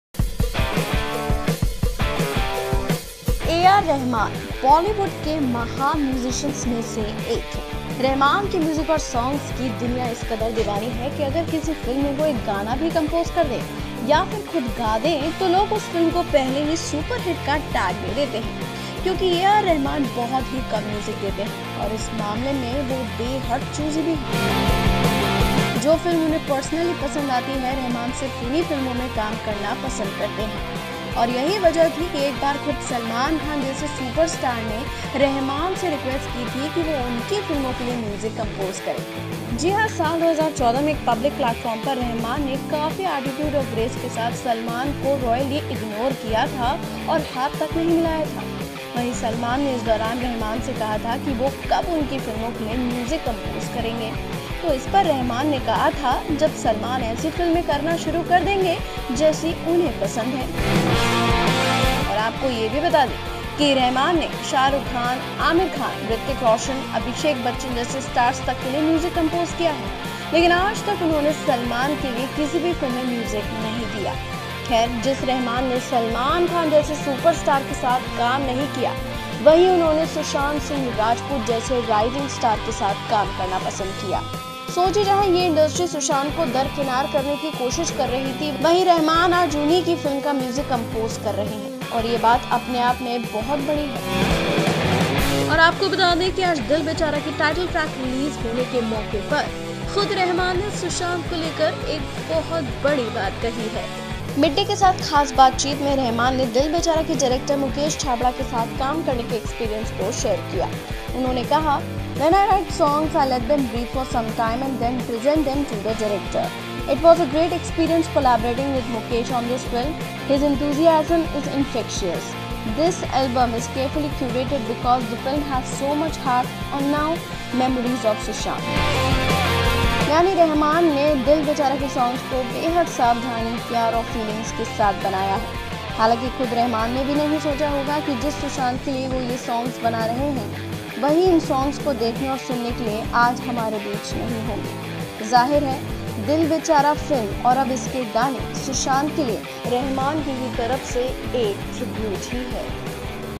AR Rahman Who Didn’t Work With Superstar Like Salman Khan Has This Is To Say For Sushant SinghRajput
AR Rahman Who Didn’t Work With Superstar Like Salman Khan Has This Is To Say For Sushant Singh Rajput